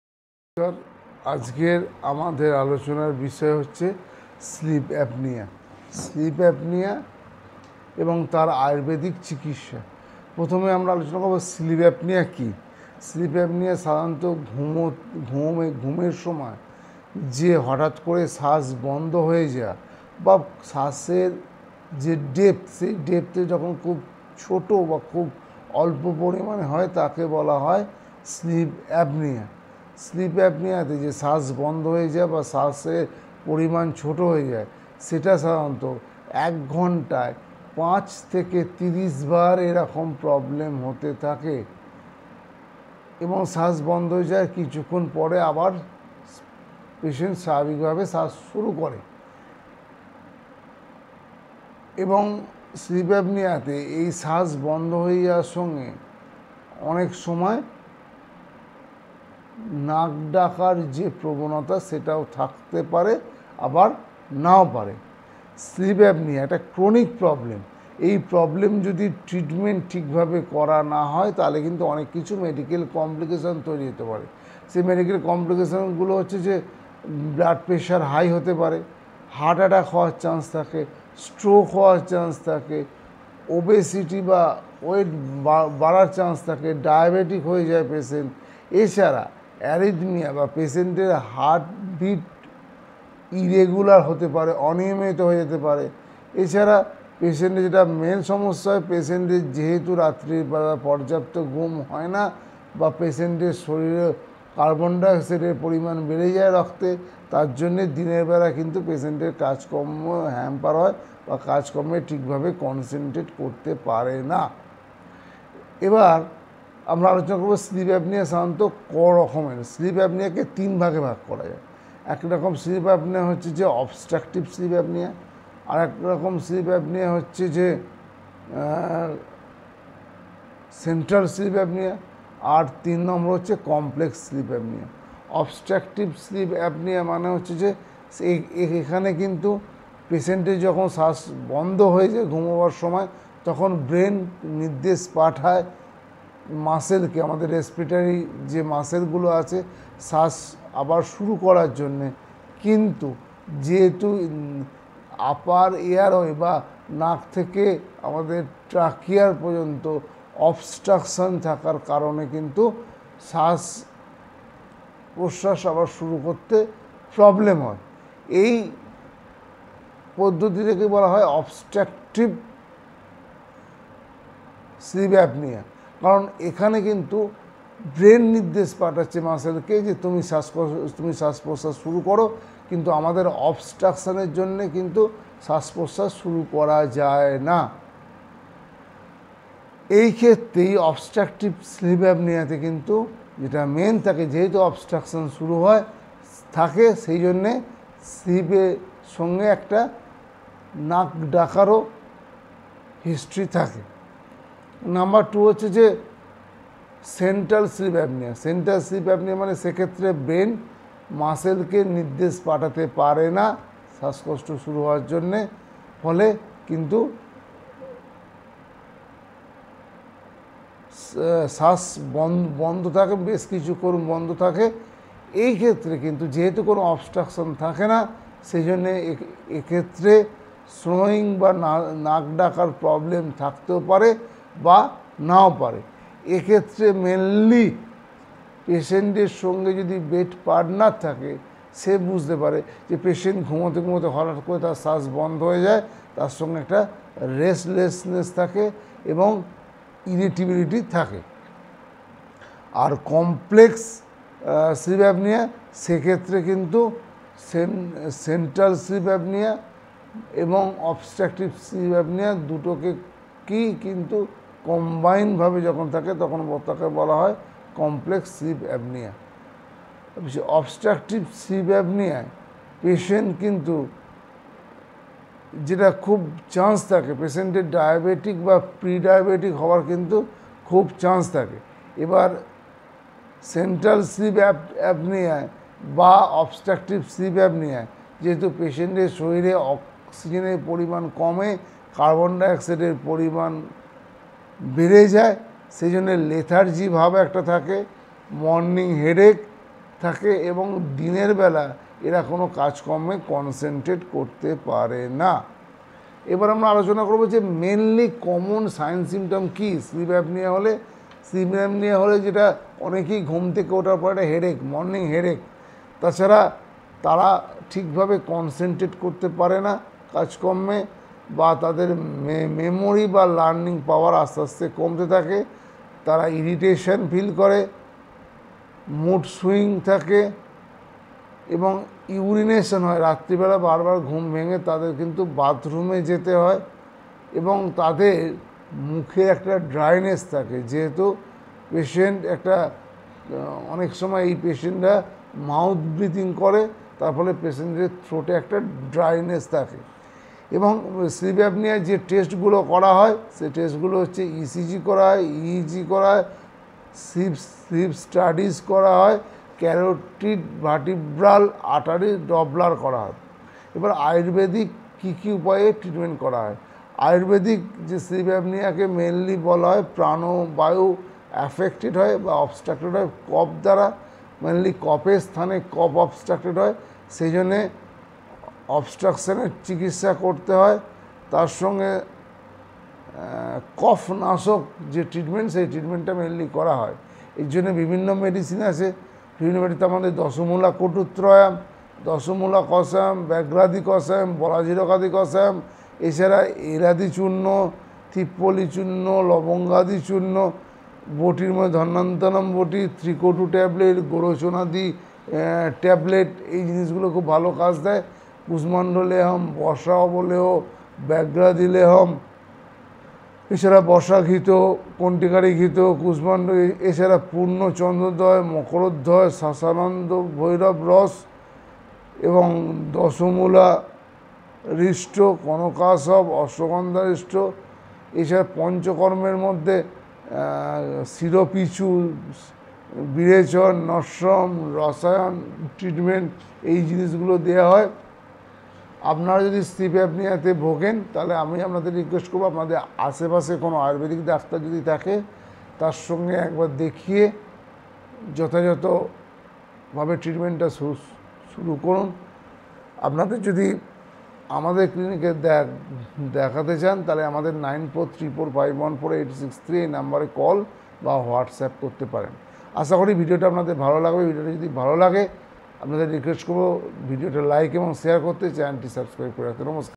sleep apnea and its ayurvedic treatment
Sleep apnea means pause or shallow breathing during sleep. Pause of breath occur 5-30 times in hour.